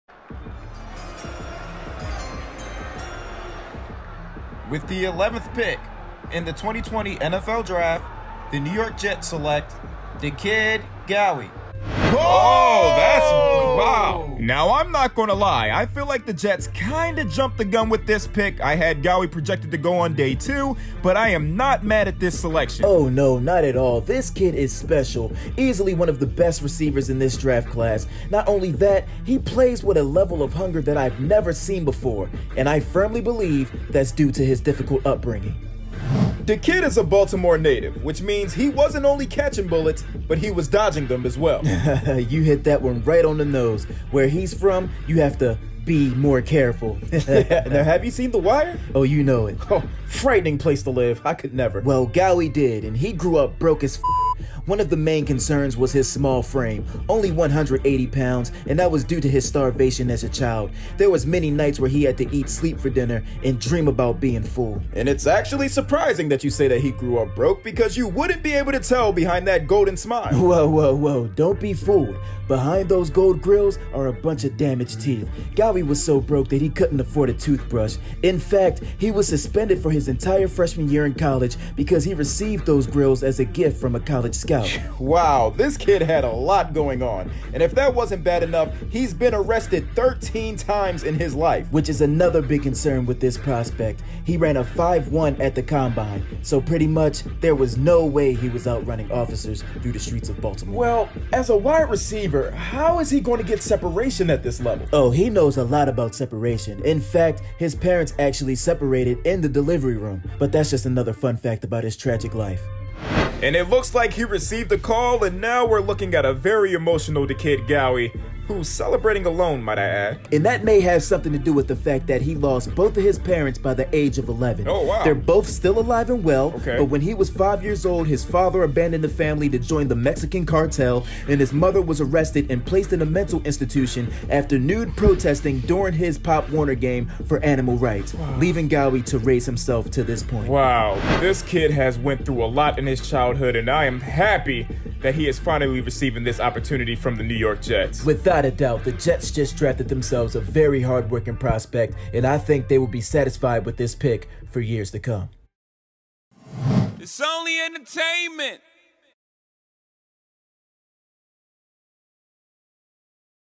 NFL Draft Be Like...
During the NFLDraft, I can guarantee that each year, we'll hear a bunch of sad stories about the prospects. Here's a parody of what I'm talking about!